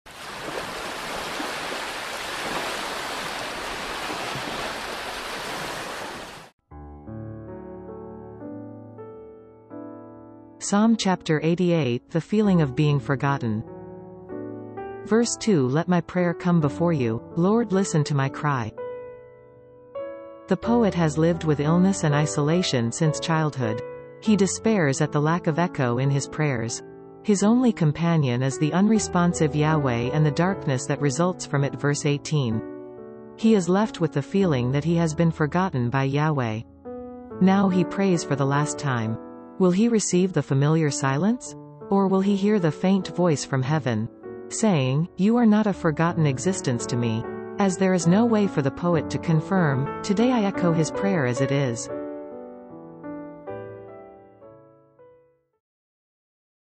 Psalm 88: The Feeling of Being Forgotten
Verse 2: Let my prayer come before you, Lord; listen to my cry.

The poet has lived with illness and isolation since childhood. He despairs at the lack of echo in his prayers. His only companion is the unresponsive Yahweh and the darkness that results from it (Verse 18). He is left with the feeling that he has been forgotten by Yahweh. Now he prays for the last time. Will he receive the familiar silence? Or will he hear the faint voice from heaven saying, "You are not a forgotten existence to me"? As there is no way for the poet to confirm, today I echo his prayer as it is.

Discover daily moments with God's Word, and through prayer, unlock the power of reflection and application in your life.